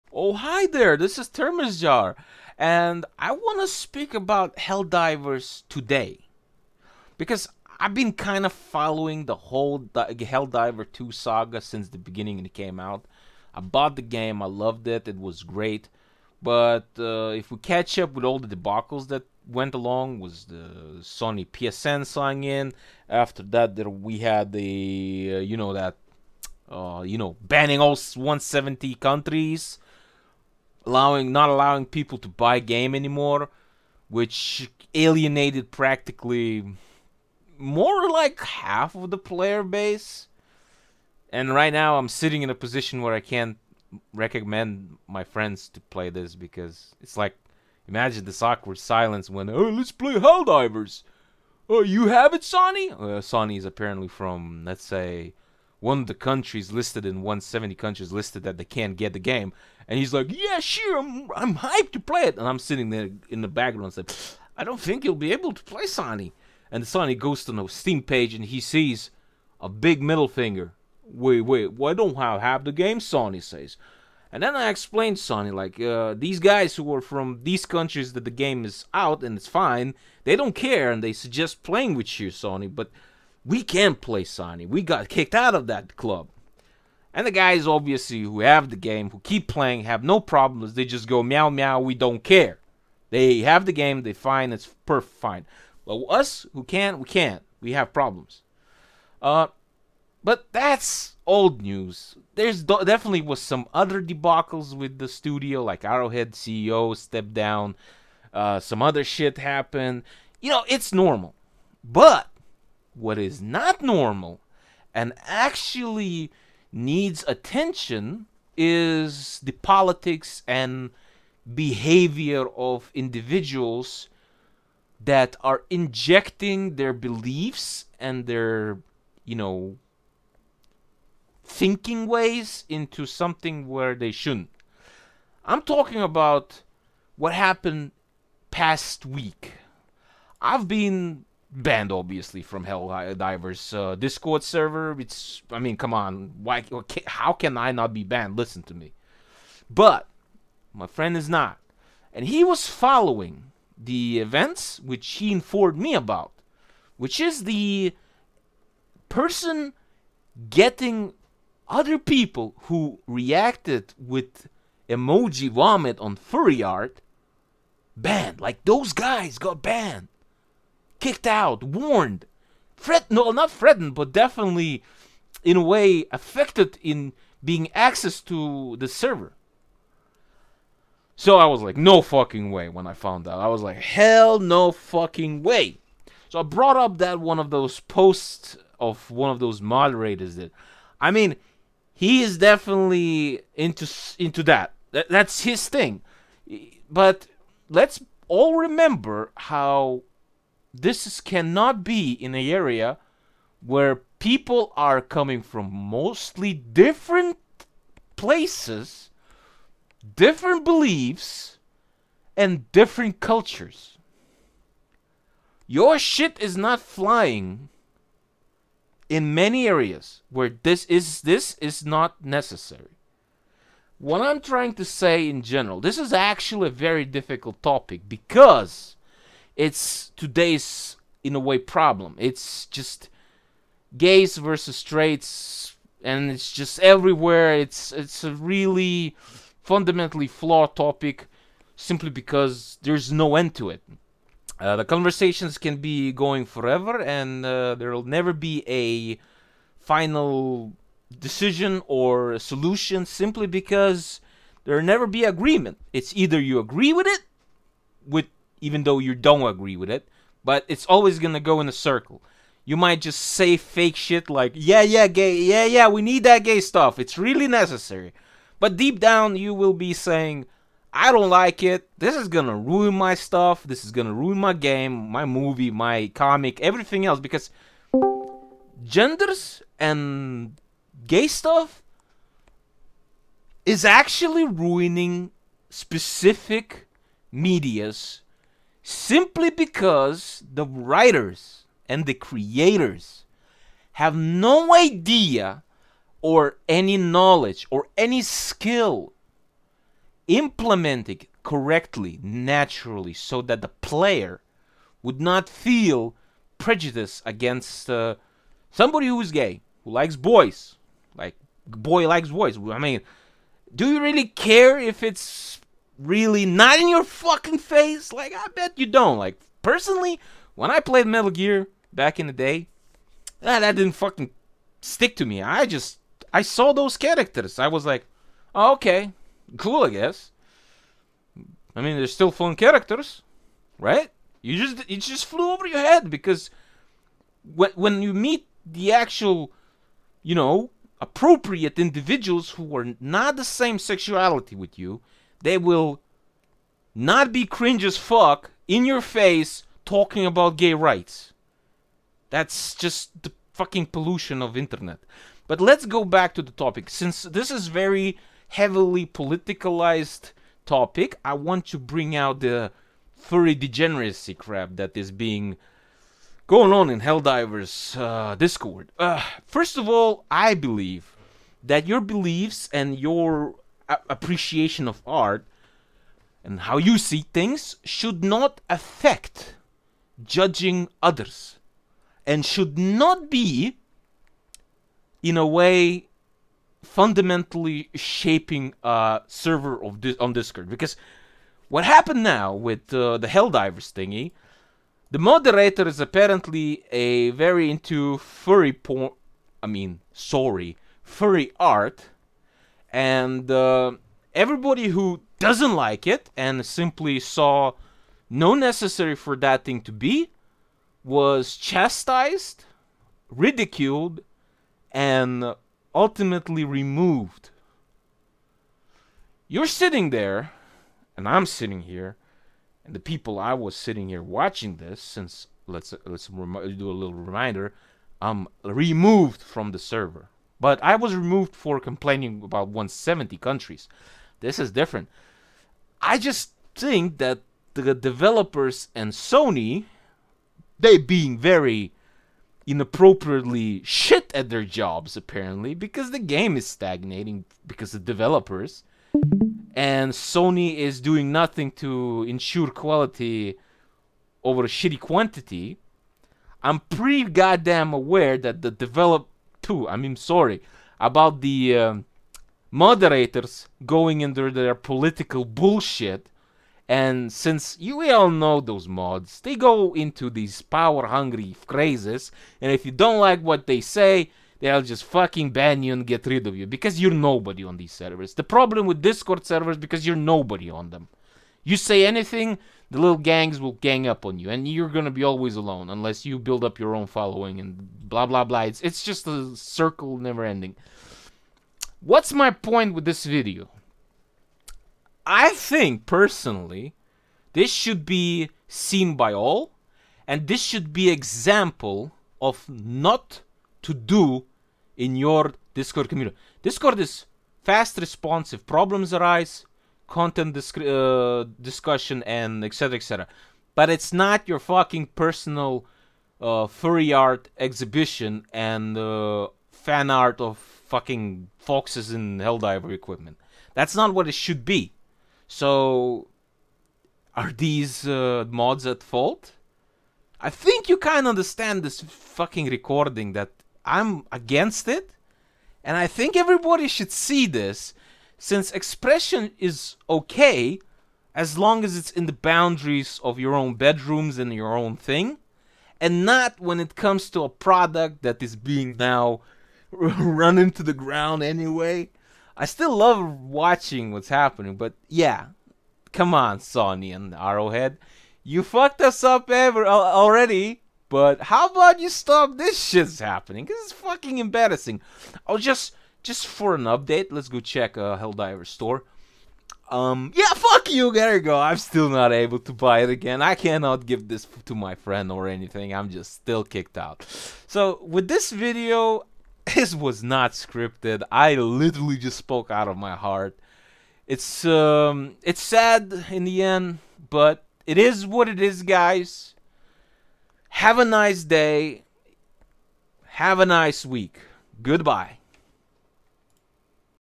Helldivers 2: Furry posting caused DRAMA and made a artifical BANNING wave BROKE me into a rant.
Rant about highway too hell called Helldirvers 2.